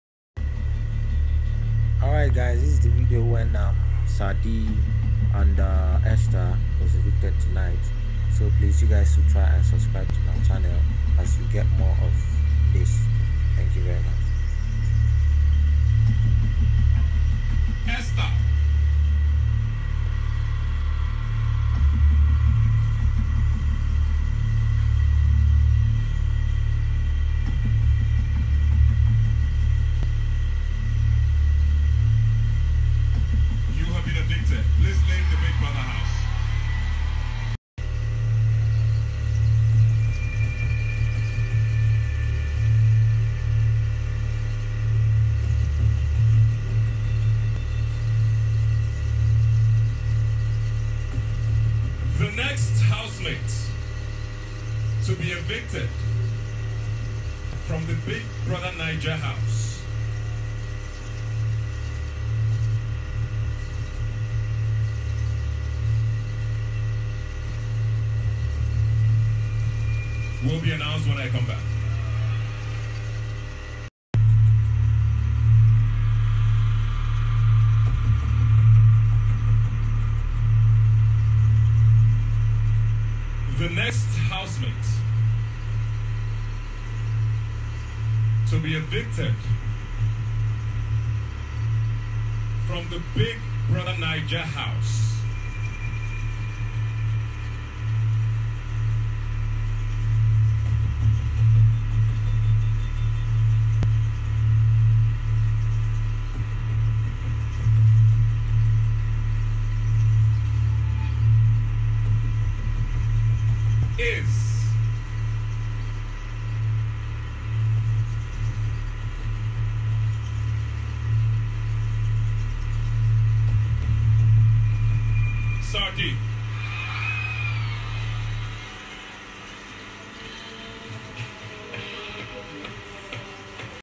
Moments Tonight as More Housemates (Esther & Sir Dee) Were Evicted.
In Tonight's episode , two housemates(Esther & Sir Dee) were evicted from the house. Watch and Enjoy